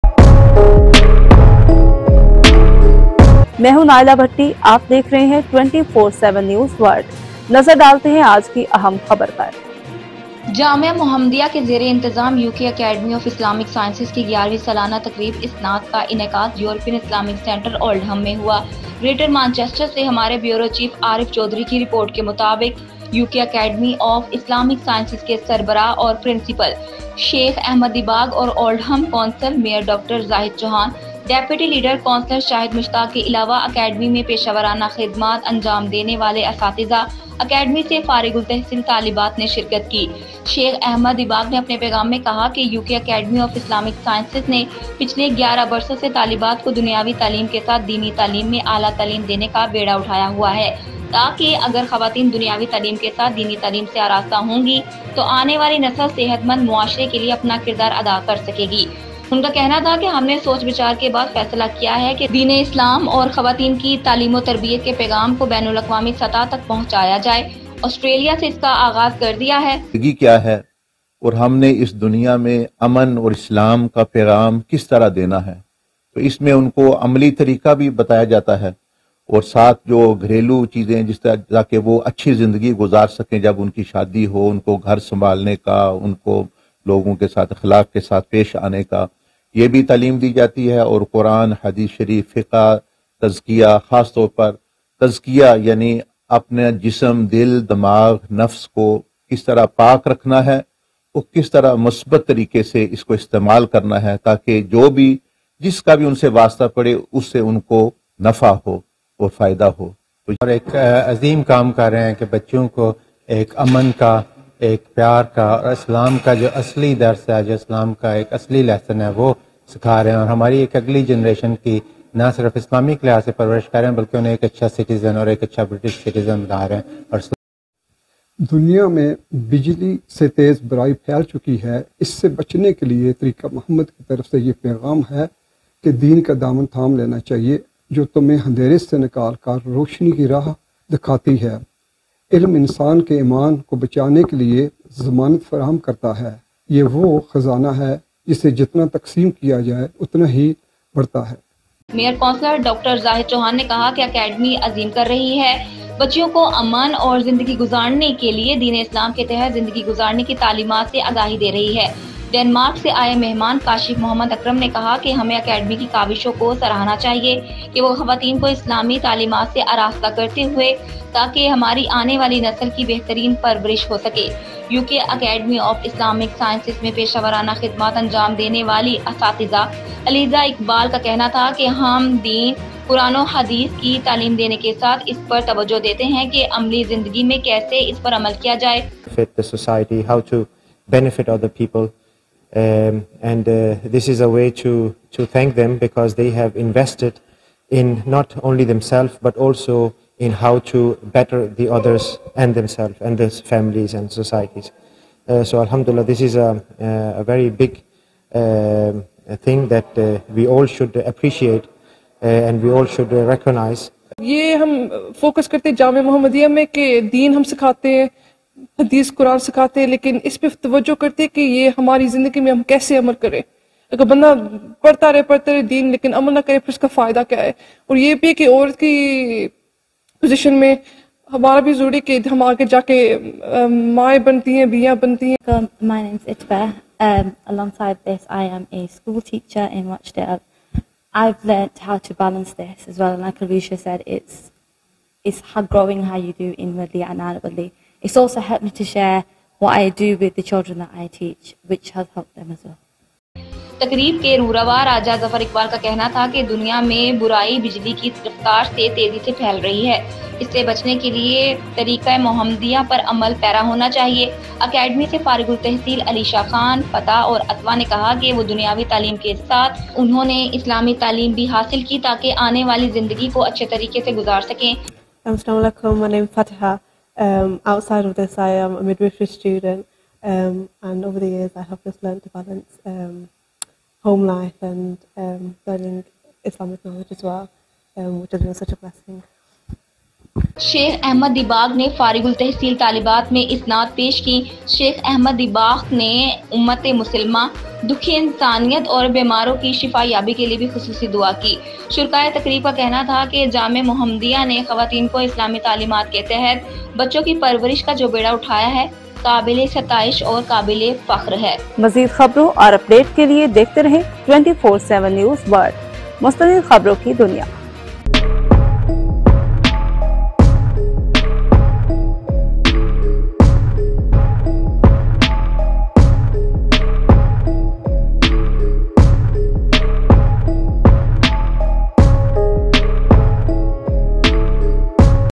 جامعہ محمدیہ کے زیر انتظام یوکے اکیڈمی آف اسلامک سائنسز کی گیارہویں سالانہ تقریب اسناد کا انعقاد
یورپین اسلامک سنٹر اولڈہم میں ہوا ۔ گریٹر مانچسٹر سے ہمارے بیورو چیف عارف چوہدری کی رپورٹ کے مطابق یوکے اکیڈمی آف اسلامک سائنسز کے سربراہ و پرنسپل شیخ احمد دباغ اور اولڈہم کونسل مئیر ڈاکٹر زاہد چوہان ،ڈپٹی لیڈر کونسلر شاہد مشتاق کے علاوہ اکیڈمی میں پیشہ وارانہ خدمات انجام دینے والے اساتذہ ، اکیڈمی سے  فارغ التحصیل طالبات نے شرکت کی ۔شیخ احمد دباغ نے اپنے پیغام میں کہا کہ یوکے اکیڈمی آف اسلامک سائنسزنے پچھلے گیارہ برسوں سے طالبات کو دنیاوی تعلیم کے ساتھ دینی تعلیم میں اعلی تعلیم دینے کا بیڑا اٹھایا ہوا ہے تاکہ اگر خواتین دنیاوی تعلیم کے ساتھ دینی تعلیم سے آراستہ ہوں گی تو آنے والی نسل صحت مند معاشرے کے لیے اپنا کردار ادا کر سکے گی ۔ انہوں نے کہا ہم نے سوچ بچار کے بعد فیصلہ کیا ہے دین اسلام اور خواتین کی تعلیم و تربیت کے پیغام کو بین الاقوامی سطح تک پہنچایا جائے آسڑیلیا سے اسکا آغاز کر دیا ہے ۔ مئیر کونسلر ڈاکٹر زاہد چوہان نے کہا کہ اکیڈمی عظیم کر رہی بچیوں کو امن اور زندگی گزارنے کے لیے دین اسلام کے تحت زندگی گزارنے کی تعلیمات سے آگاہی دے رہی ہے۔ ڈنمارک سے آئے مہمان کاشف محمد اکرم نے کہا کہ ہمیں اکیڈمی کی کاوشوں کو سراہنا چاہیے کہ وہ خواتین کو اسلامی تعلیمات سے آراستہ کرتے ہیں تاکہ ہماری آنے والی نسل کی بہترین پرورش کر سکیں ۔یوکے اکیڈمی آف اسلامک سائنسز میں پیشہ وارانہ خدمات انجام دینے والی اساتذہ علیزہ اقبال کا کہنا تھا کہ ہم دین قرآن و احادیث کی تعیلم دینے کے ساتھ اس پر توجہ دیتے ہیں کہ عملی زندگی میں کیسے اس پر عمل کیا جائے ۔تقریب کے روح رواں راجہ ظفر اقبال نے کہا کہ دنیا میں برائ بجلی کی رفتا جیسی پھیل رہی ہے اس سے بچنے کے لیے طریقہ محمدیہ پر عمل پیرا ہونا چاہیے۔ اکیڈمی سے فارغ التحصیل علیشا خان ، فتحہ اور اطوا نے کہا کہ وہ دنیاوی تعلیم کے ساتھ انہوں نے اسلامی تعیلم بھی حاصل کی تاکہ آنے والی زندگی کو اچھے طریقے سے گزار سکیں۔ شیخ احمد دباغ نے فارغ التحصیل طالبات میں اسناد پیش کیں ۔ شیخ احمد دباخ نے امت مسلمہ ،دکھی انسانیت اور بیماروں کی شفایابی کے لیے خصوصی دعا کی۔